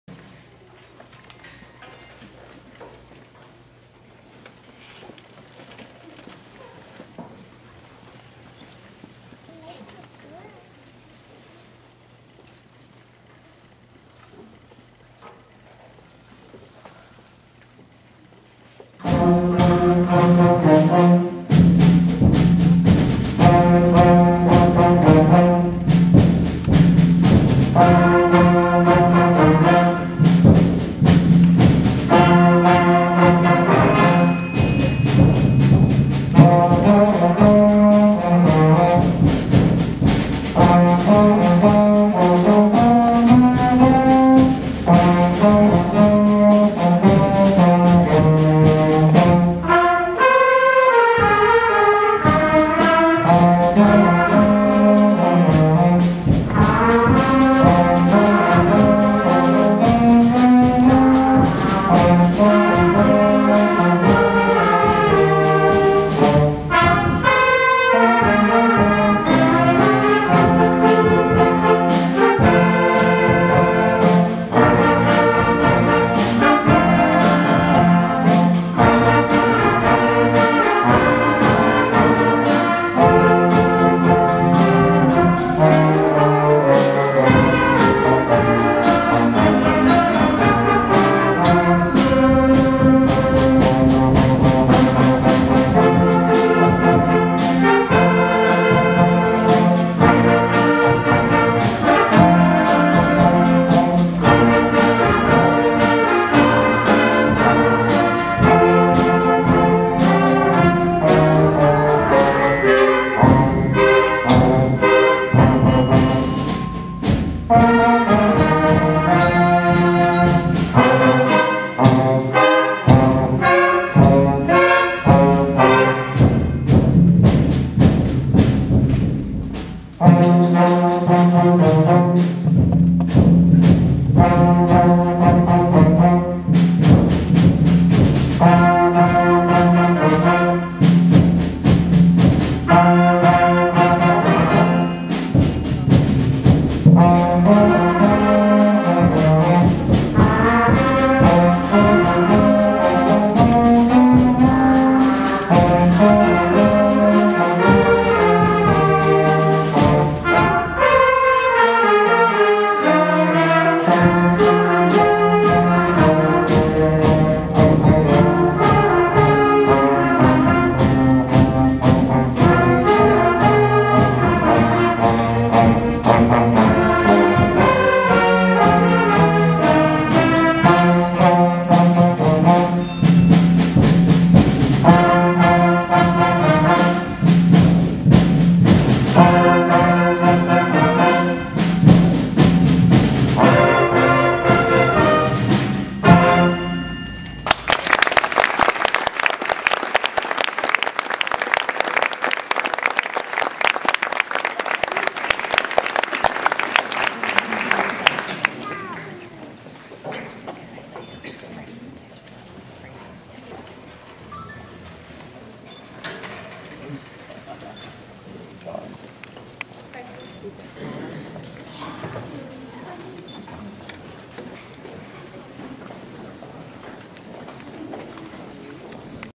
Josh - Amity Middle School Band 3
Josh playing saxophone in the Amity Middle School band.